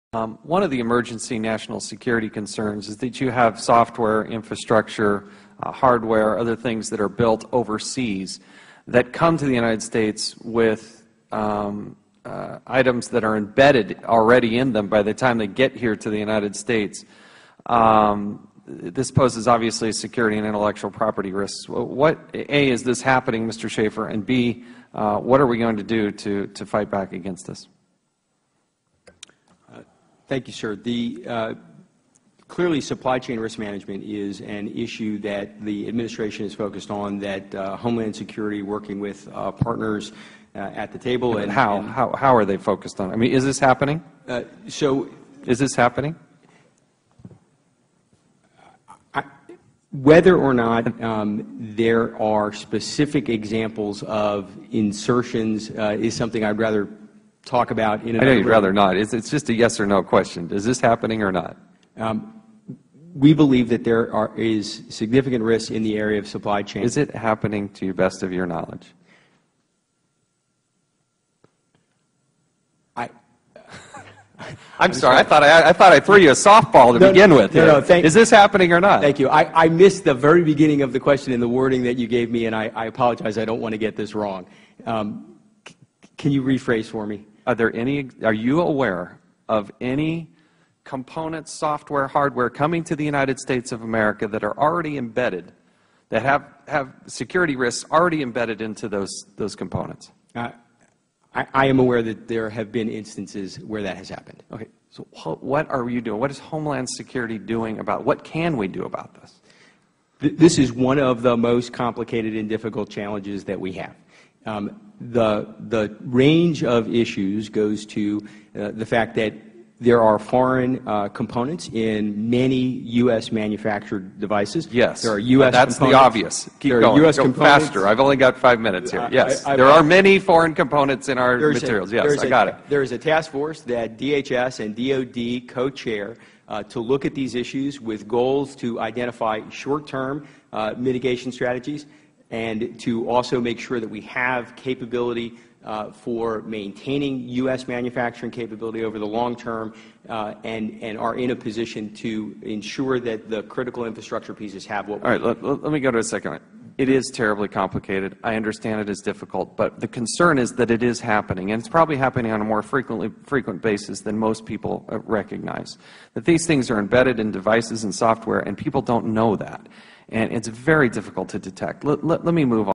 Jason Chaffetz Gets Gangsta On Guy From DHS
Rep. Jason Chaffetz questions DHS Assistant Secretary Gregory Schaffer during a hearing titled, "Cybersecurity: Assessing the Nation's Ability to Address the Growing Cyber Threat," held on July 7th, 2011.

During the hearing Mr. Schaffer tried to avoid answering a question asked of him by Rep, Chaffetz who did not back down until finally receiving an answer after multiple attempts.  It was refreshing to see one of our representatives due what was necessary to get a legitimate answer to an important question.